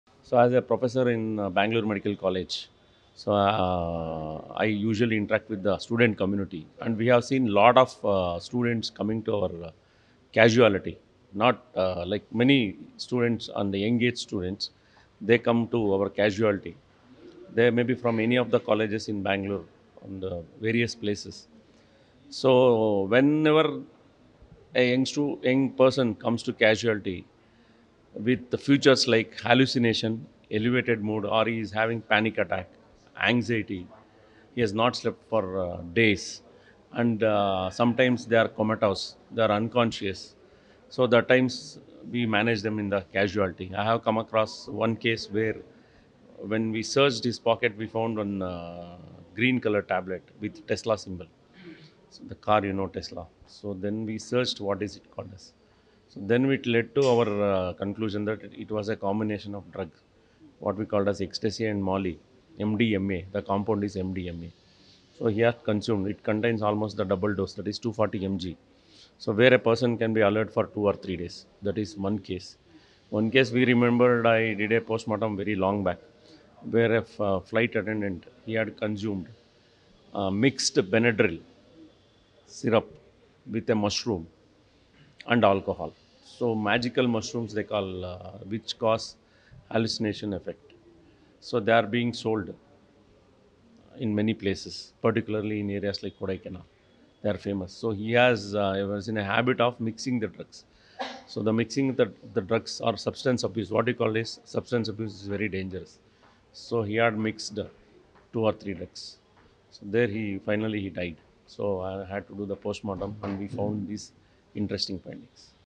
Drug abuse day: Know how harmful MDMA, magical mushrooms are? | NewsFirst Prime
On the occasion of International Day Against Drug Abuse, Dr S. Venkata Raghava, Joint Director, Directorate of Medical Education, Government of KarnatakaProfessor & Head, Department of Forensic Medicine & Toxicology, Bangalore Medical College & Research Centre, Bangalore, shares his thoughts on how youngsters can fall prey to drugs like MDMA and magical mushrooms. The event was organised by the forensic science department of Jain University (Deemed-to-be)